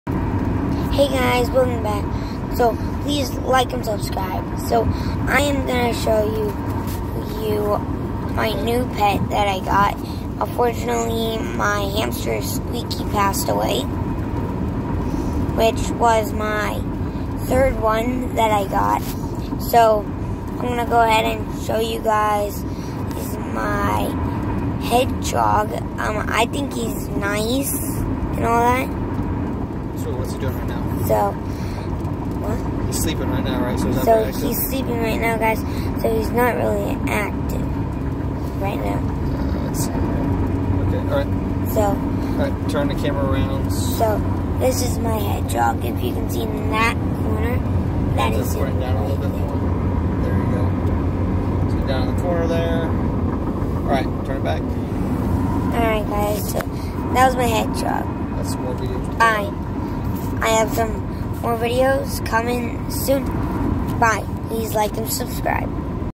New pet hedgehog
Unfortunately our latest pet Squeaky passed away this week so we have gotten a hedgehog which is what Thomas J was told he would be able to get once his hamster passed.